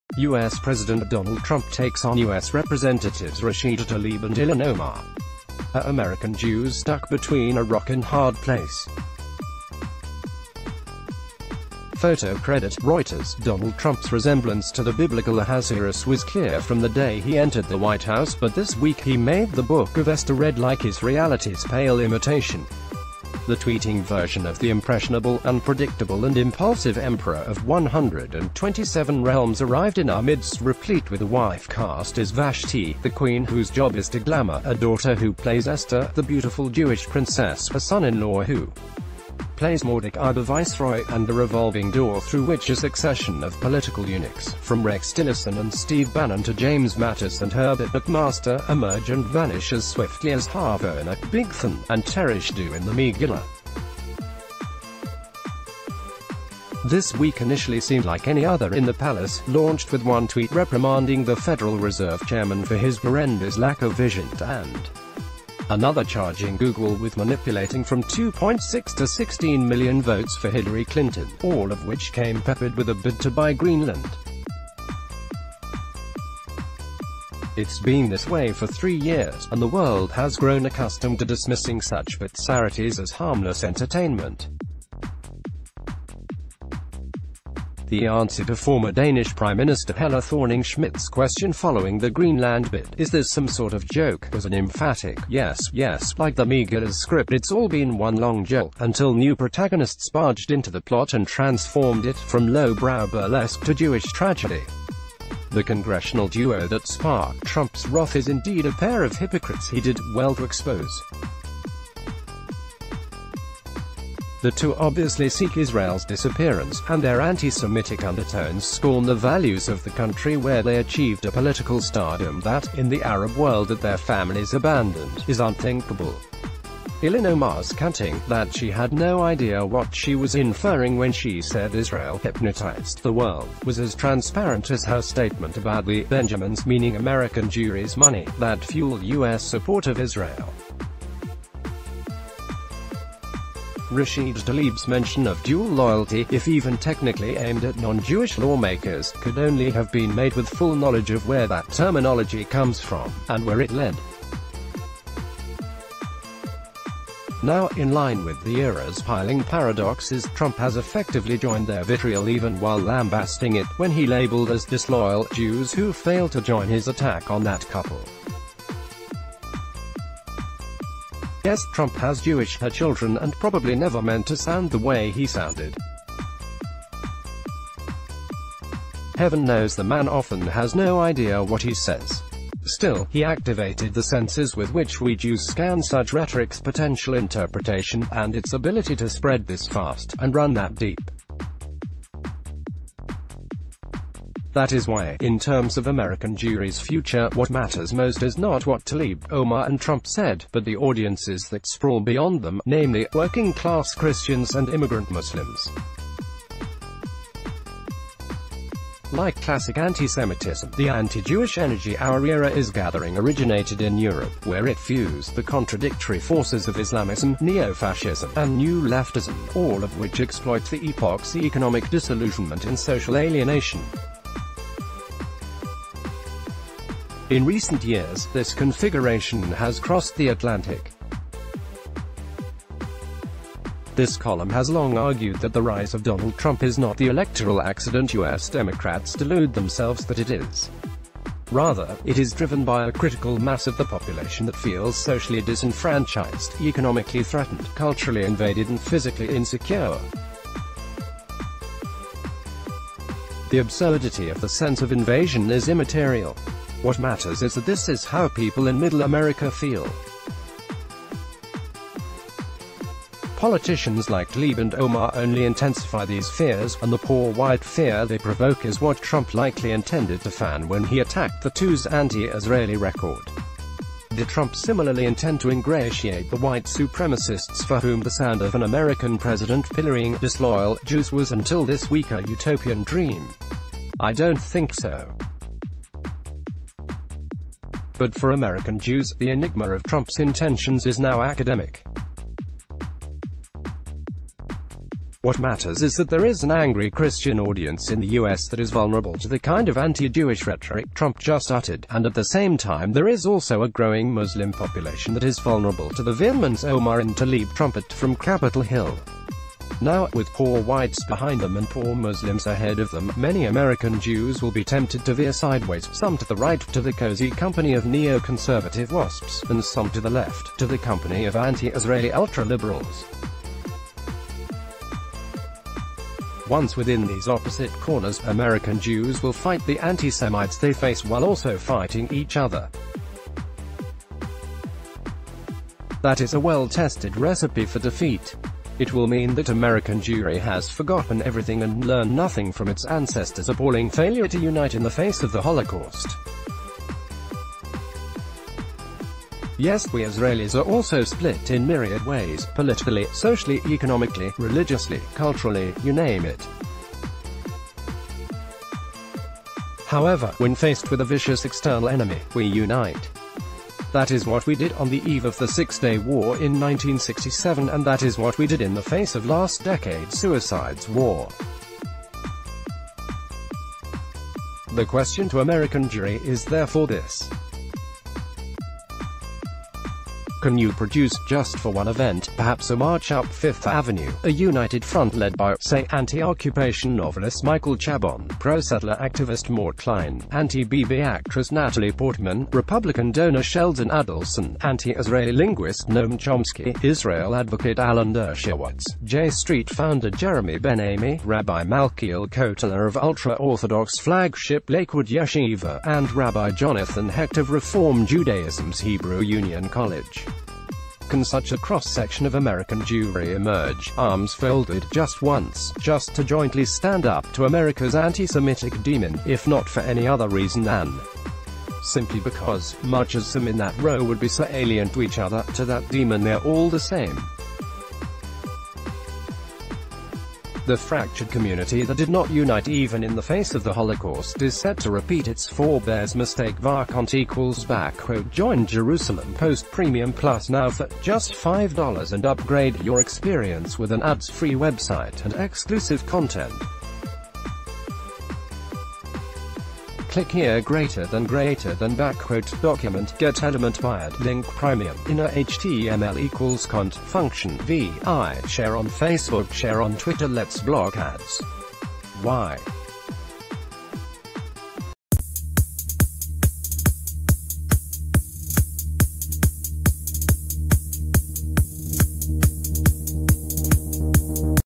As antisemitic threat grows, can US Jews unite? | USA Politics News
US PRESIDENT Donald Trump takes on US Representatives Rashida Tlaib and Ilhan Omar. Are American Jews stuck between a rock and hard place?. (photo cre...